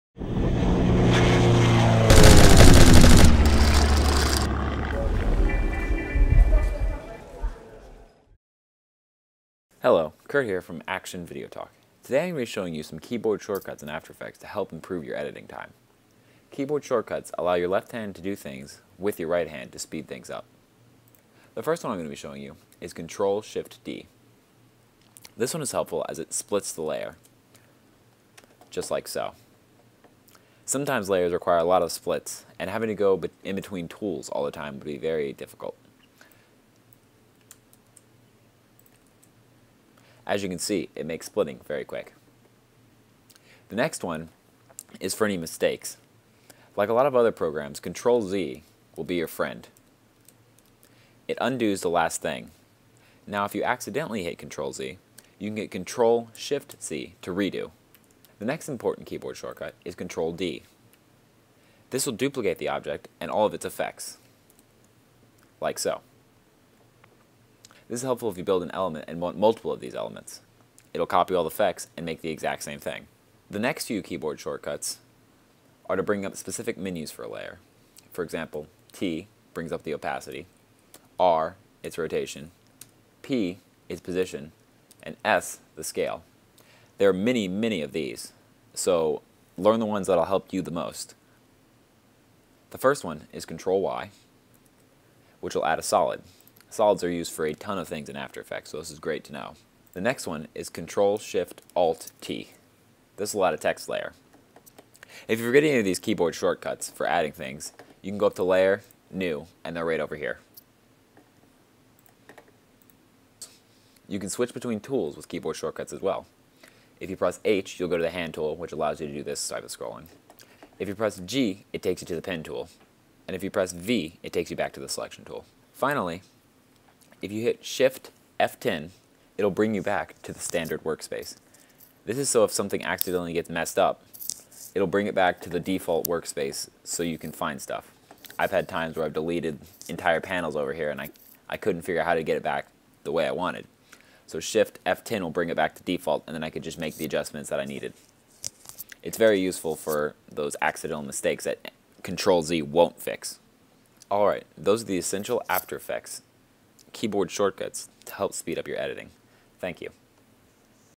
After Effects CC Keyboard Shortcuts (Hotkeys) Tutorial
Today I am going to be showing some Keyboard shortcuts in After Effects to improve your Editing time! The keyboard shortcuts I go over are below! Thanks, and many more videos will be coming, If you have a suggestion on my video, or would like to suggest a tutorial I should do, don't be afraid to leave a comment.

Ctrl + Shift + D = Cut a Layer
Ctrl + Z = Undo
Ctrl + Shift + Z = Redo
Ctrl + D = Duplicate

T = Opacity
R = Rotation
P = Position
S = Scale

Ctrl + Y = New Solid
Ctrl + Shift + Alt + T = Add Text Layer

H = Hand Tool
G = Pen Tool
V = Selection Tool

Shift + F10 = Reset "Standard WorkSpace"